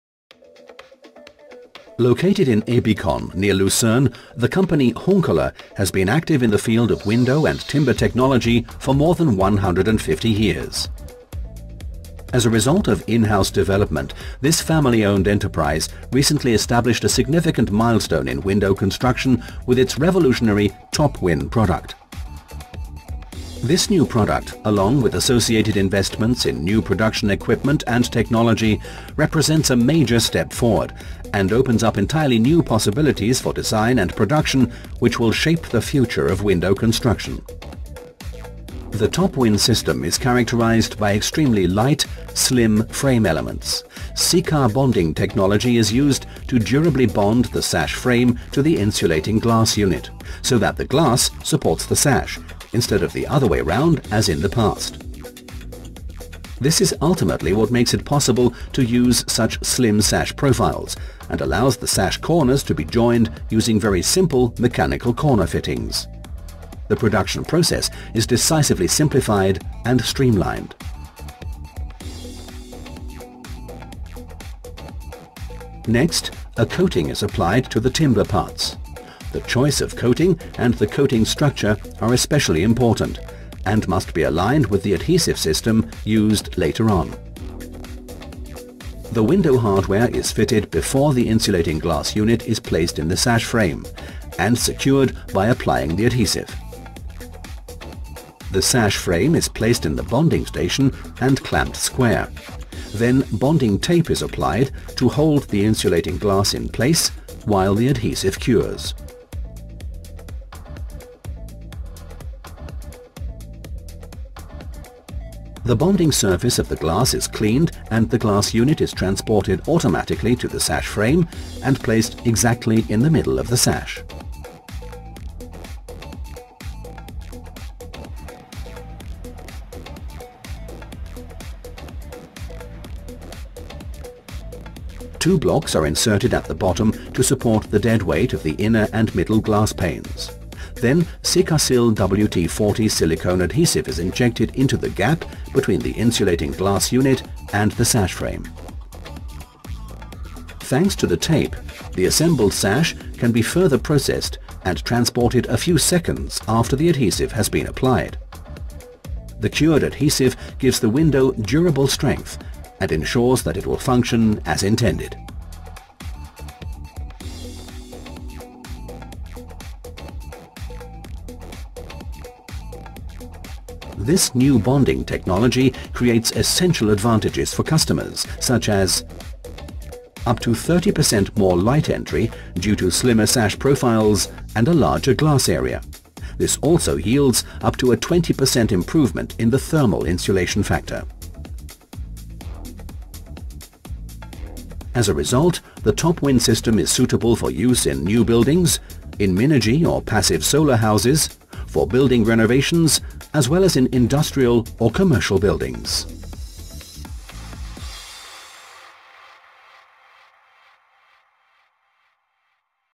SIka Вклейка стеклопакета в деревянное окно на клей Sikasil
На видео представлено референтное производство деревянных окон. Компания умело использует преимущества технологии вклейки стеклопакетов c помощью клея Sikasil - теперь не створка несет стеклопакет, а стеклопакет несет на себе створку. Благодаря равномерному распределению напряжения по периметру с помощью клея Sikasil створка стала тоньше, отпала необходимость применения внешнего штапика и значительно сократились напряжения в угловых зонах.